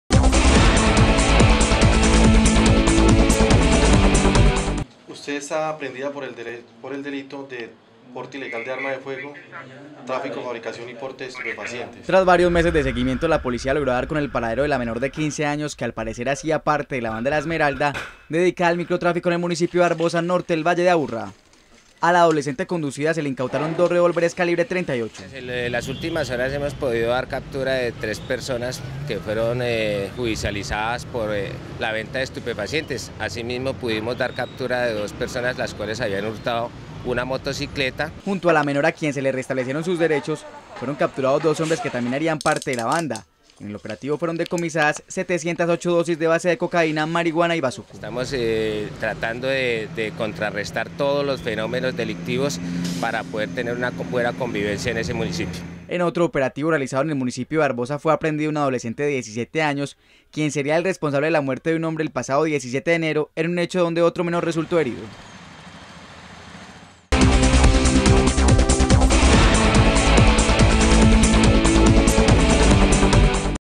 Menor de 15 años fue detenida por portar armas de fuego en Barbosa
Una menor de 15 años fue conducida por las autoridades en el municipio de Barbosa por portar armas de fuego.